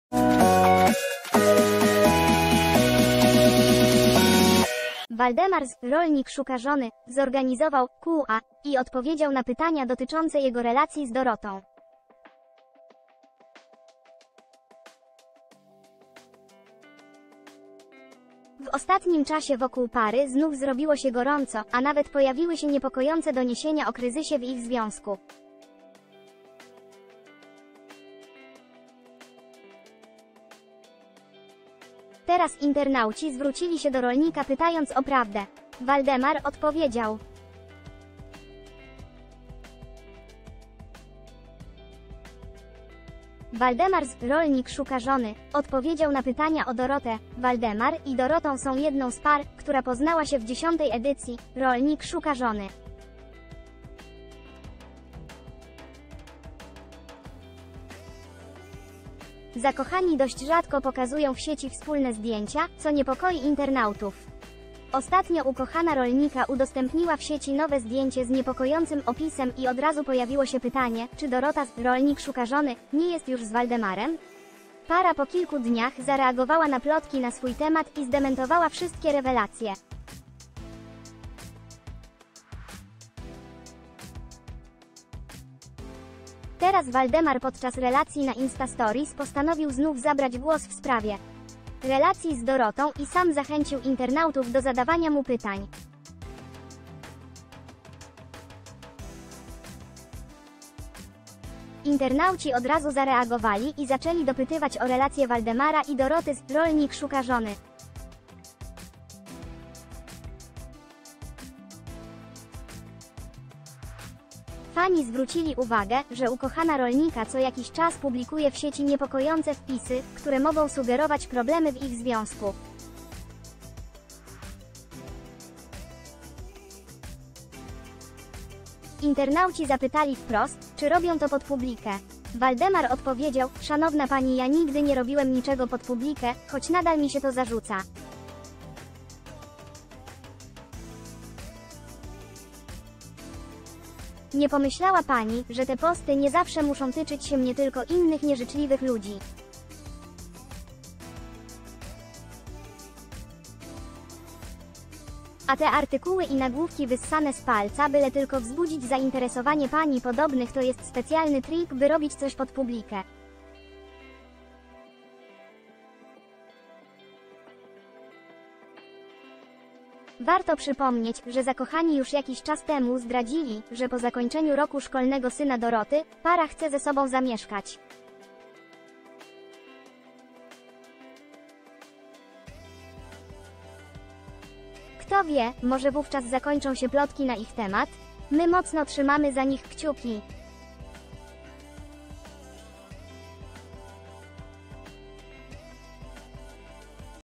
A jednak! Waldemar z "Rolnika" szczerze odpowiedział na pytanie o Dorotę. Co z ich związkiem?
Waldemar z „Rolnik szuka żony” zorganizował „Q&A” i odpowiedział na pytania dotyczące jego relacji z Dorotą. W ostatnim czasie wokół pary znów zrobiło się gorąco, a nawet pojawiły się niepokojące doniesienia o kryzysie w ich związku. Teraz internauci zwrócili się do rolnika pytając o prawdę. Waldemar odpowiedział!
Waldemar z „Rolnik szuka żony” odpowiedział na pytania o Dorotę
Waldemar i Dorotą są jedną z par, która poznała się w dziesiątej edycji „Rolnik szuka żony”. Zakochani dość rzadko pokaz  ...